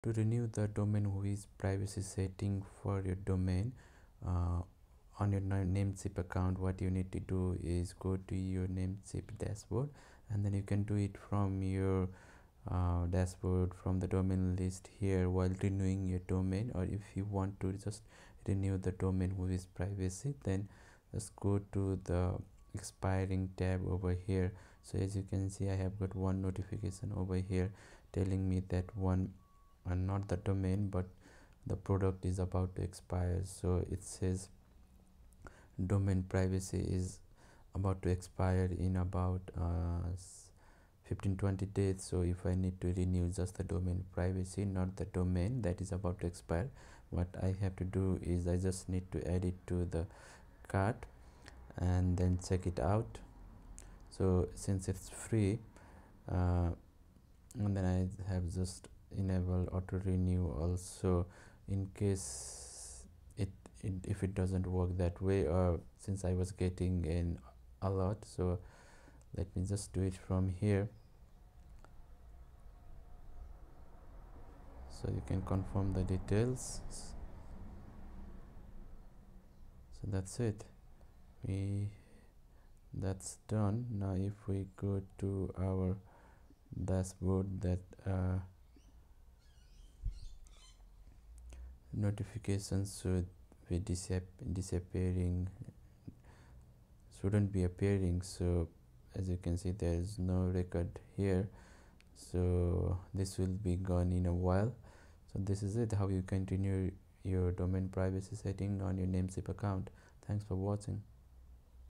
How to Manually Renew Expiring Domain Privacy with Namecheap
A short tutorial on how to manually renew a whois domain privacy for your domain when it is about to expire and you have got an alert in your Namecheap dashboard.

- Renew Expiring Domain Privacy